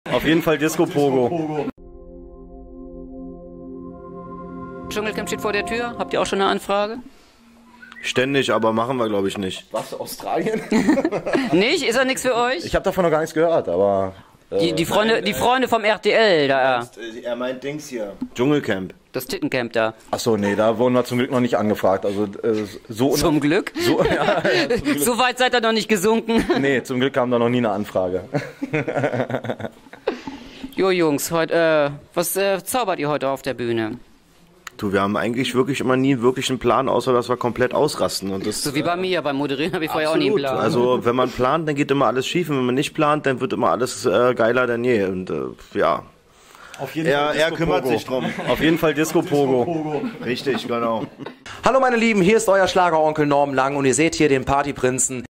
Atzen im Dschungelcamp - das sagen sie dazu
Hat RTL die Atzen angefragt? "Wir haben nie nen Plan" - so beschreiben sie ihr Bühnenprogramm... | Ich bin ein Star holt mich hier raus | Dschungelcamp